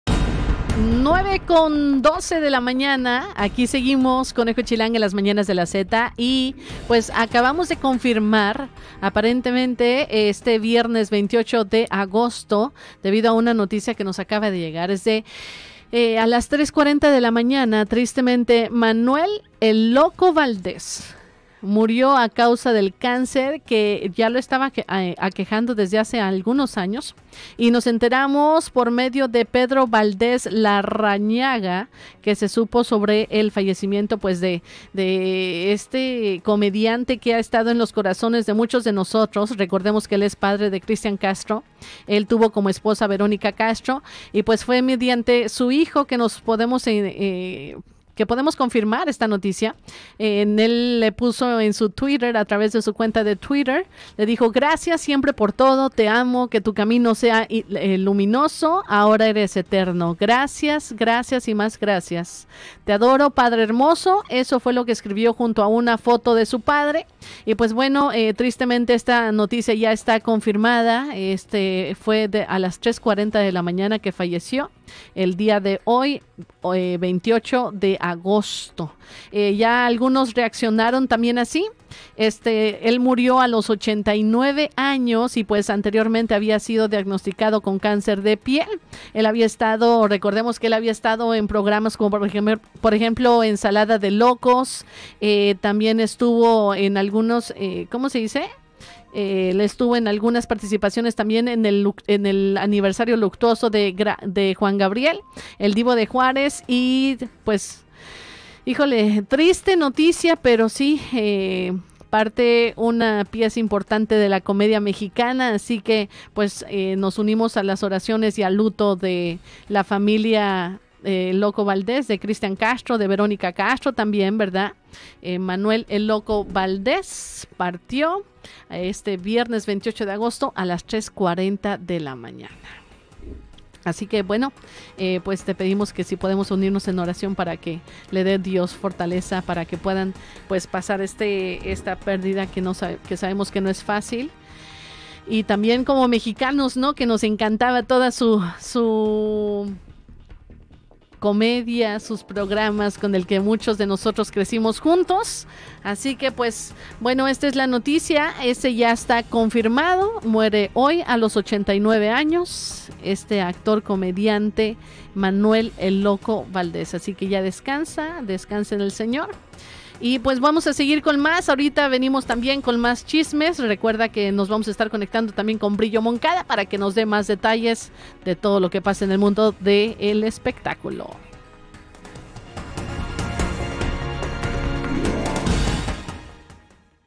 Fallece Manuel "El Loco" Valdés a los 89 años
PorSiTeLoPerdiste: Chilanga da la triste noticia que fallece el gran actor y comediante “El Loco” Valdés

Somos Tu Mejor Alternativa!